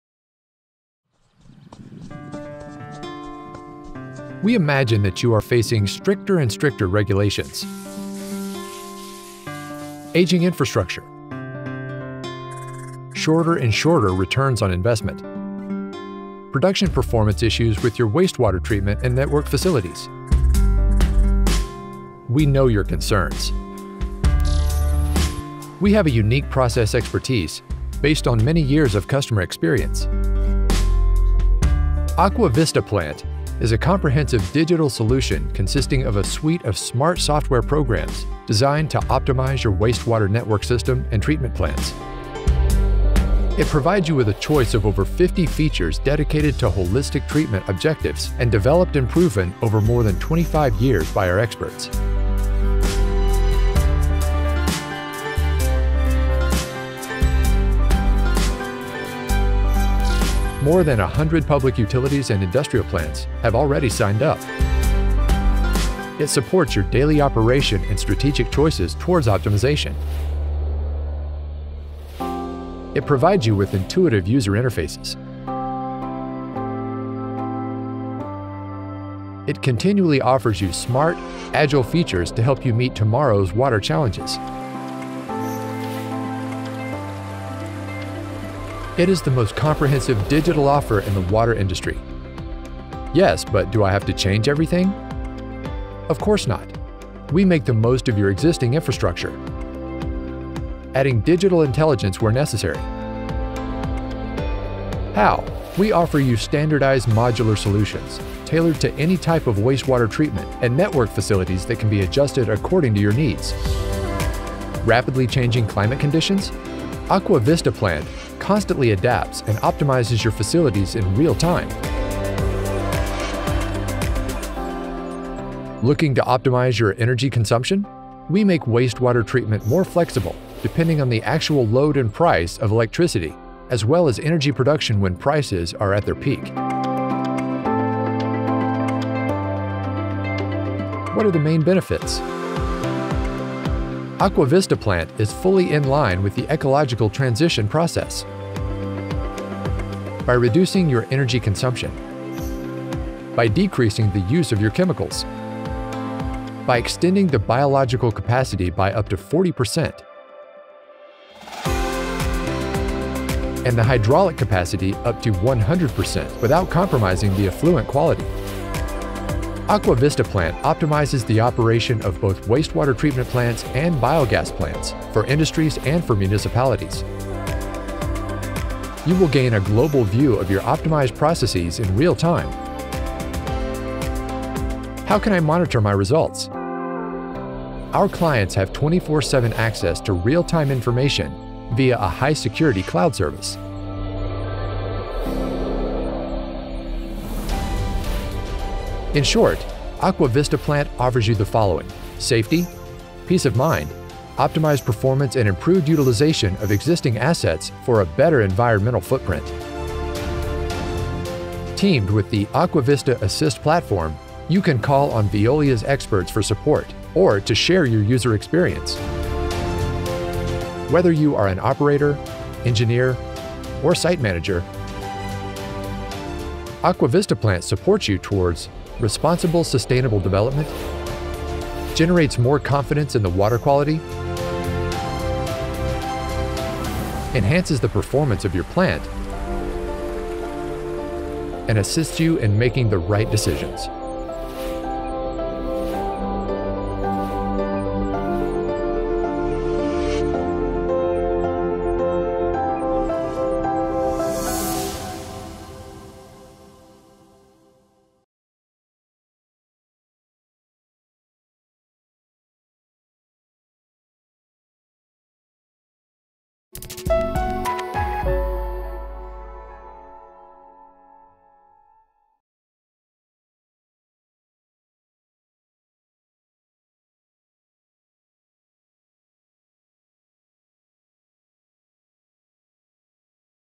Veolia Water Technologies, AQUAVISTA™ Plant
Demande du client  : Comment  expliquer les atouts de notre solution de gestion numérique des stations de traitement d'eau ?
Réponse Agence IMACOM :  Inventer une histoire, l’a tourner en Italie, rendre visible l’invisible.